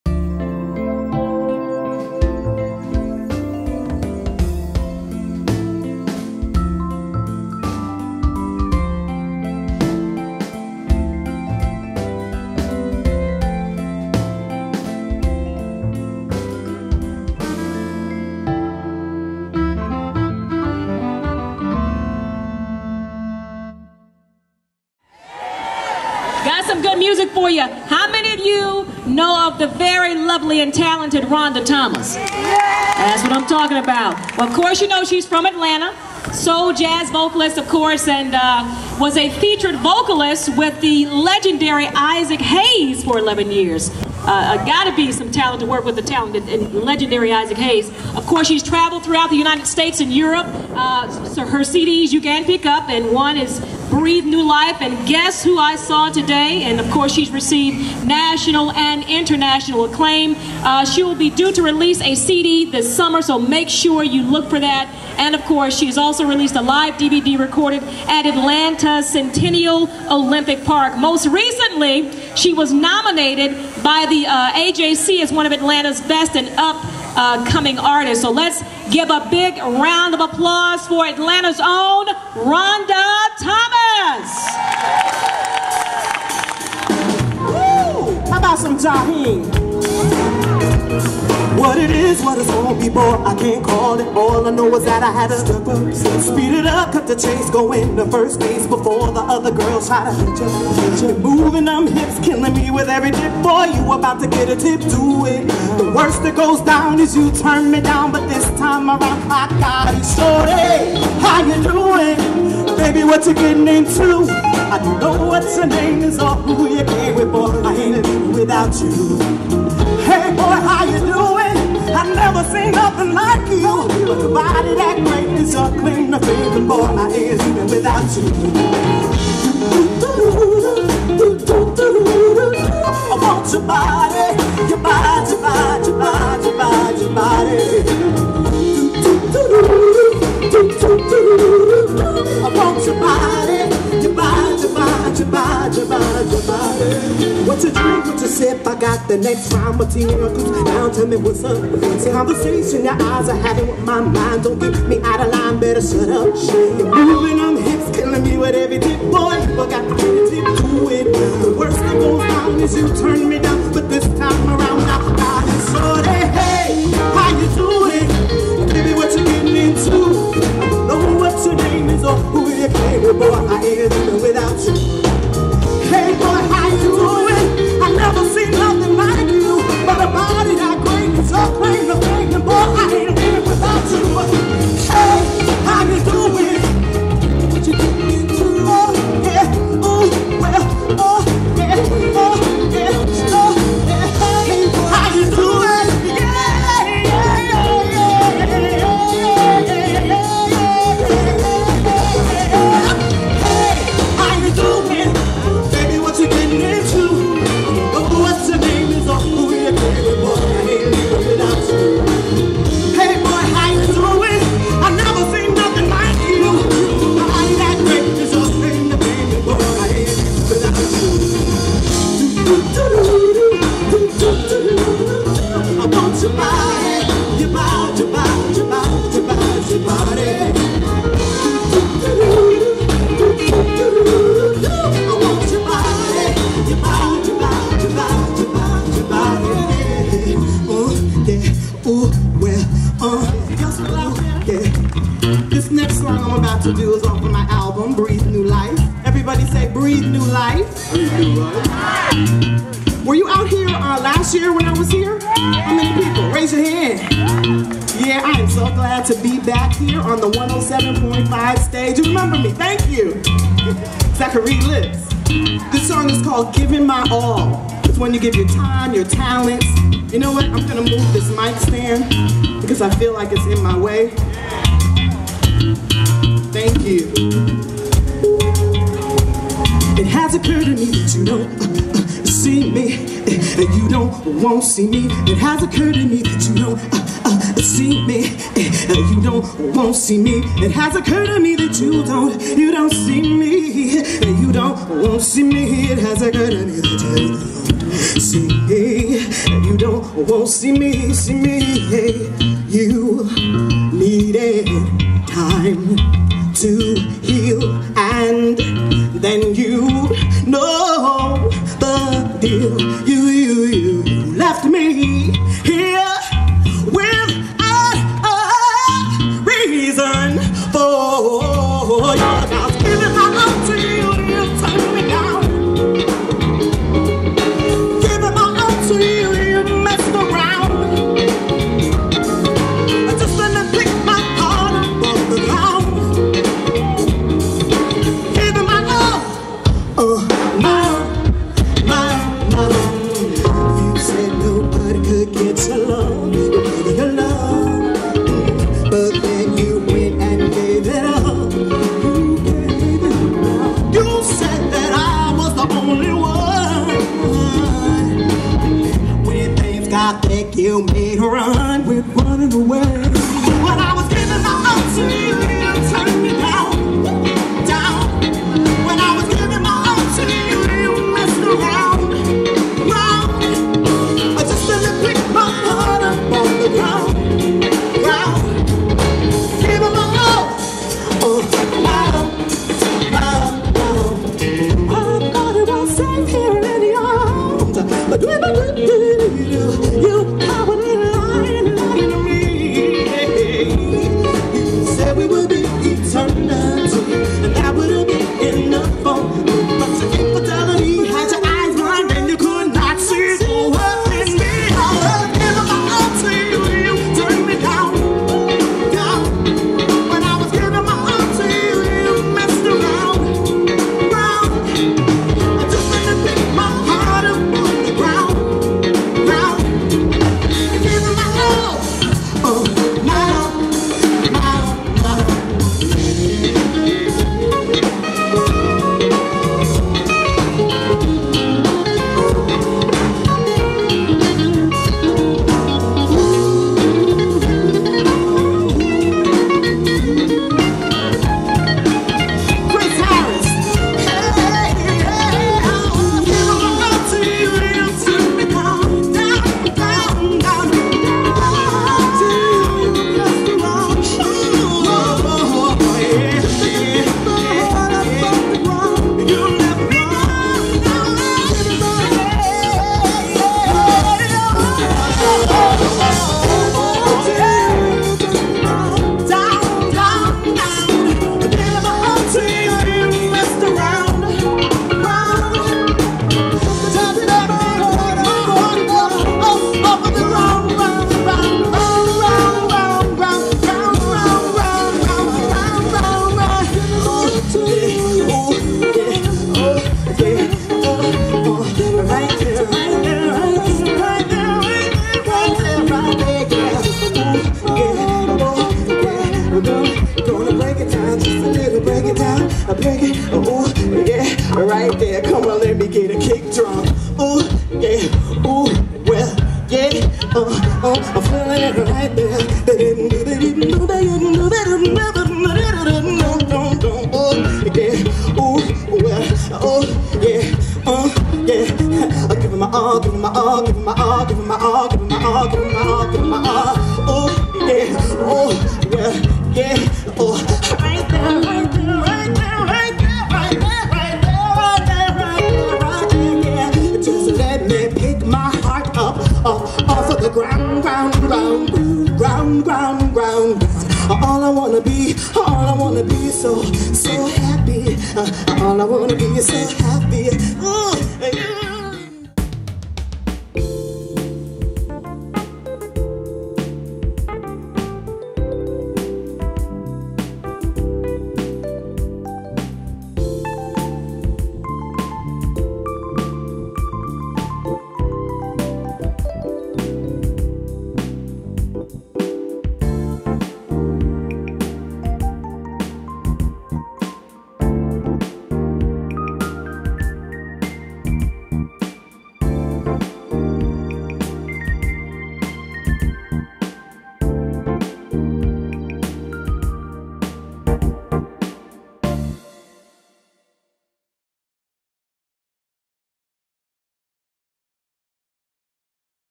Rhonda Thomas - Live Concert
Soulful Atlanta-based singer Rhonda Thomas has backed such top artist as Isaac Hayes, India Arie, Luther Vandross, Roy Ayers and many others. This performance took place at Suwanees Town Center Park presented by Majic 107.5. Video from 2010 concert.

This video is a Joyce Ray-Loper Production.
A subsidiary of JRL Enterprises, LLC of Atlanta production.